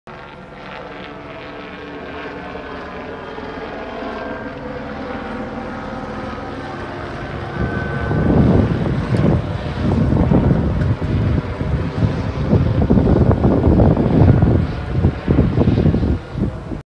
San Bernardino county sheriffs department helicopters
In this video the San Bernardino county sheriffs department helicopter did a circle